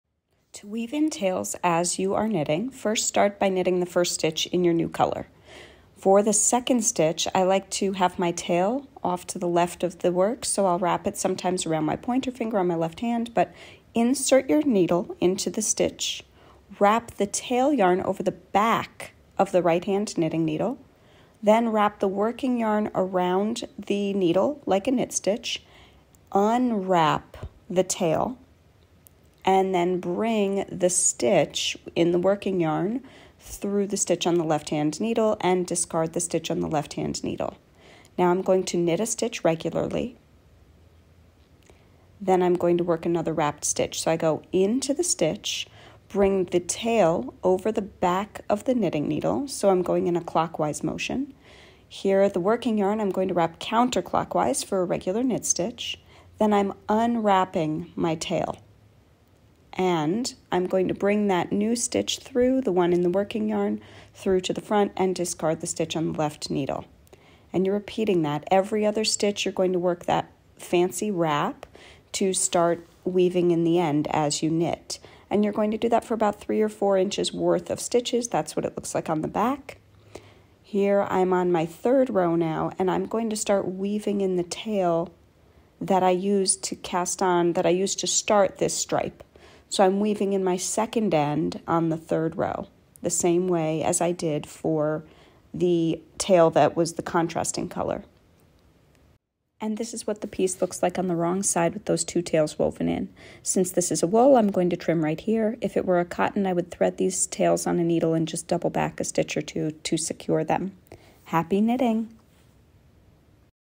Weaving in Ends as you knit - for American or English style knitters
In this Tiny Tutorial, learn how to weave in yarn tails as you knit. This video is geared towards English or American style dominant knitters. Video by knitting instructor and knitwear designer Shaina Bilow.